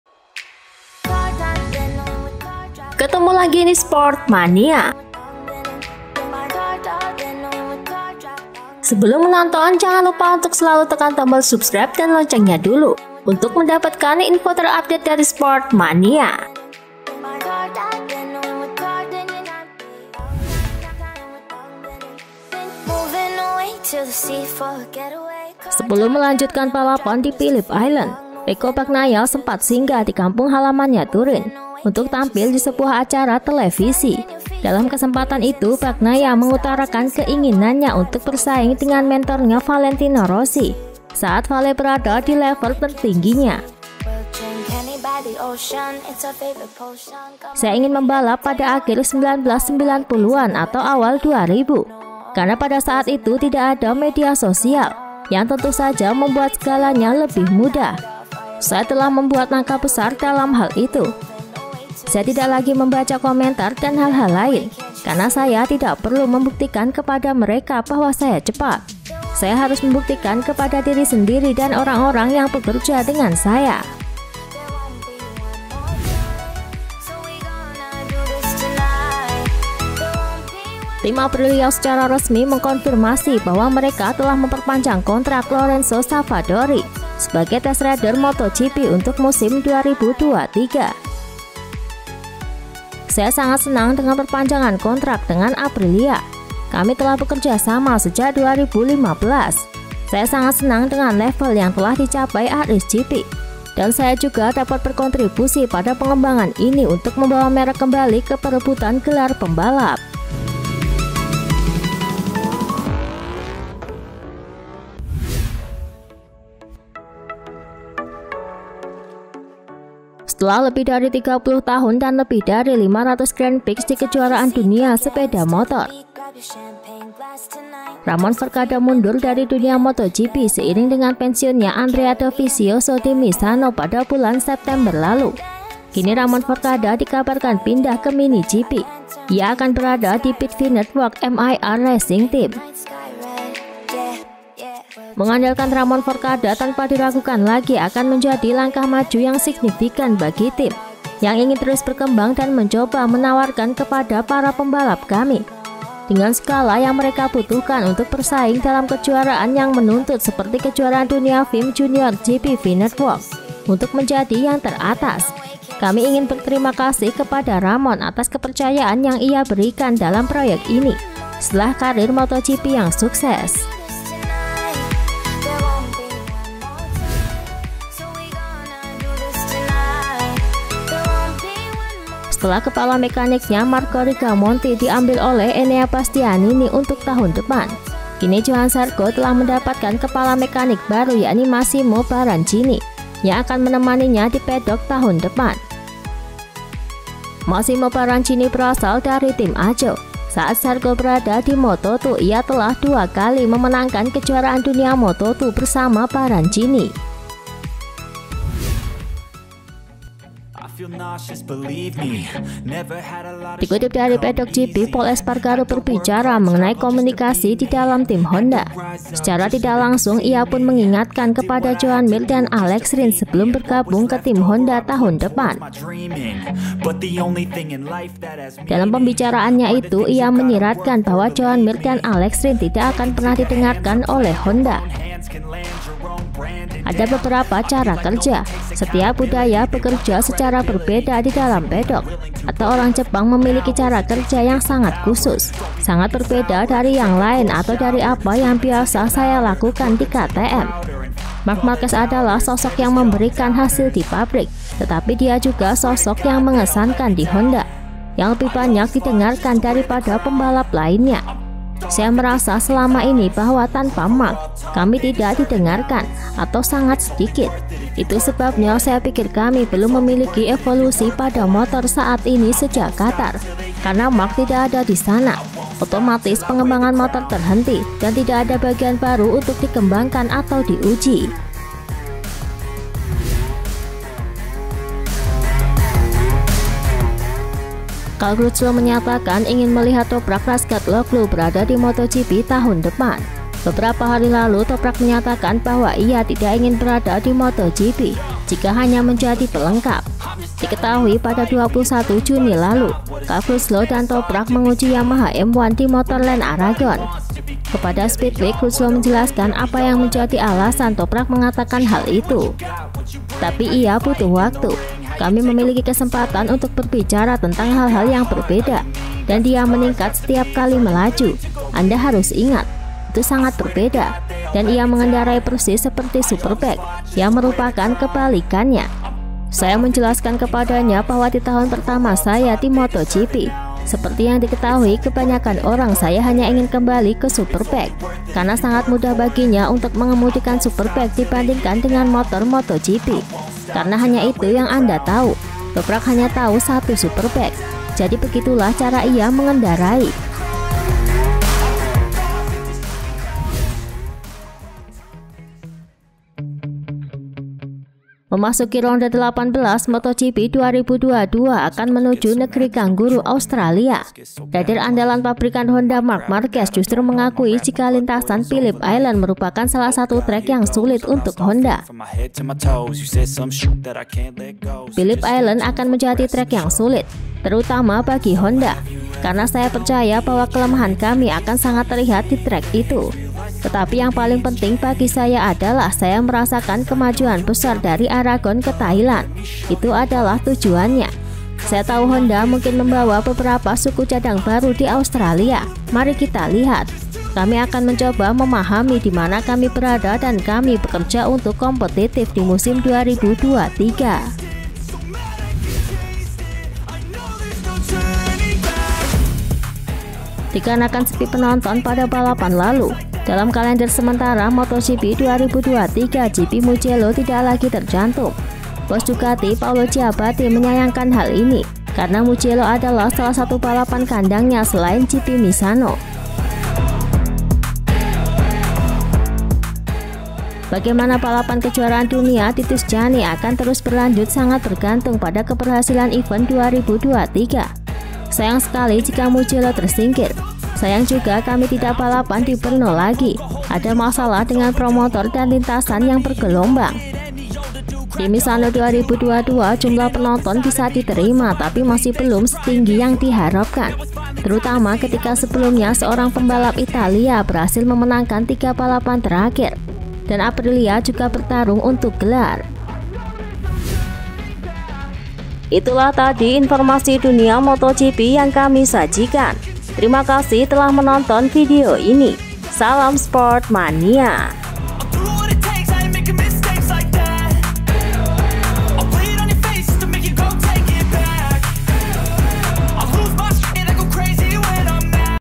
MARQUEZ:HONDA PASTI KALAH DI PHILIP ISLAND🏍POL INGATKAN MIR DAN RINS🏍FORCADA PINDAH KE MINI GP
BAGNAIA INGIN DUEL DGN VALENTINO ROSSI
LORENZO SAVADORI JADI TES RIDER APRILIA
RAMON FORCADA PINDAH KE MINI GP
KEPALA MEKANIK BARU ZARCO
KETIKA POL ESPARGARO MENGINGATKAN MIR DAN RINS
CAL CRUTCHLOW BERI NASEHAT KE TOPRAK
MARQUEZ: HONDA PASTI KALAH DI PHILIP ISLAND
BOS DUCATI KECEWA GP MUGELLO DIHAPUS